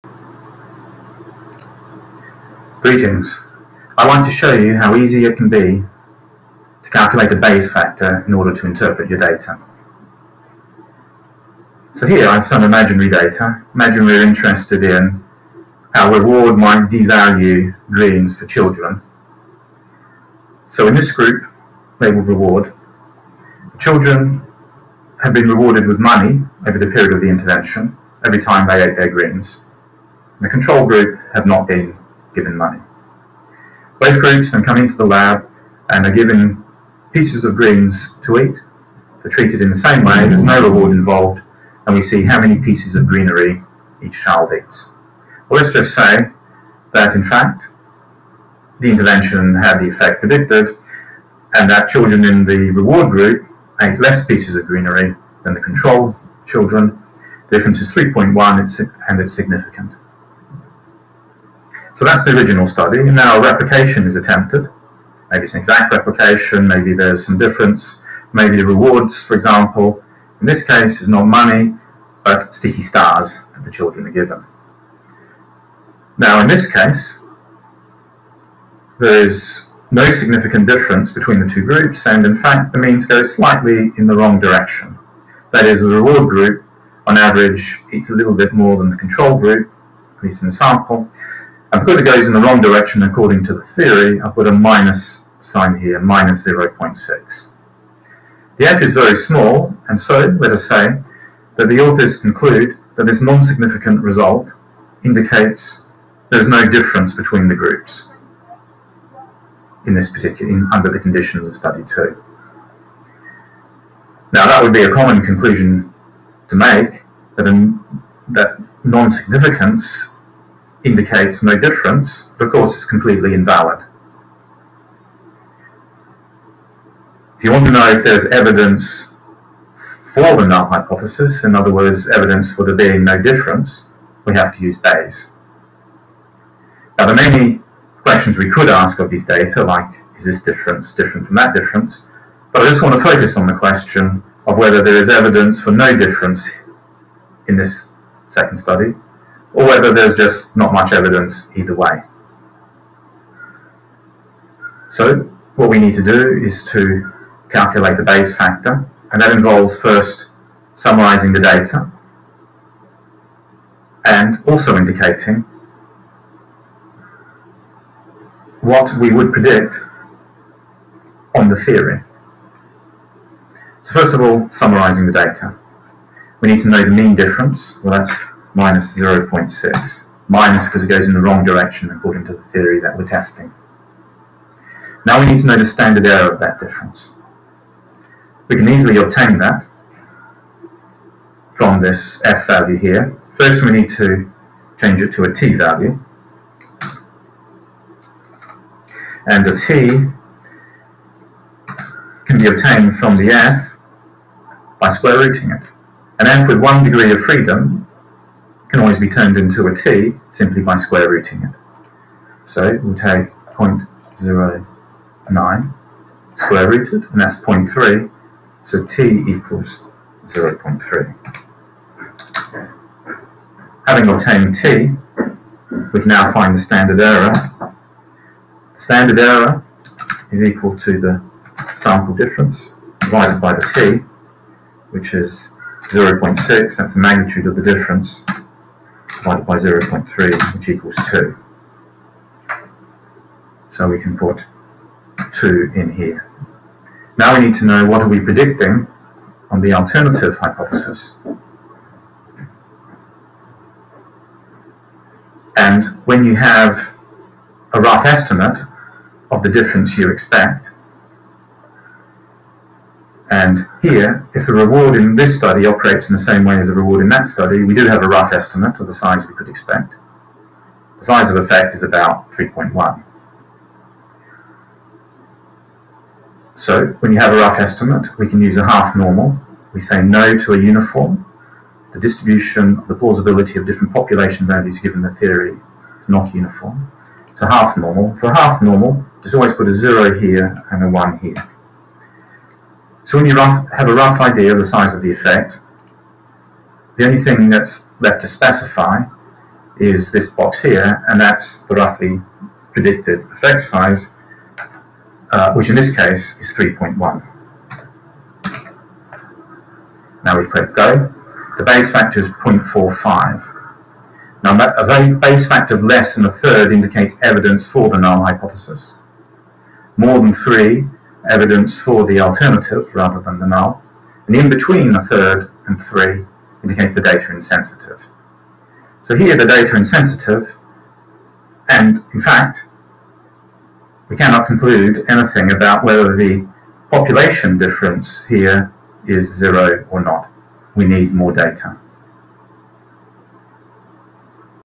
How to calculate a Bayes factor
A 5-minute example of using the Bayes factor calculator freely available at this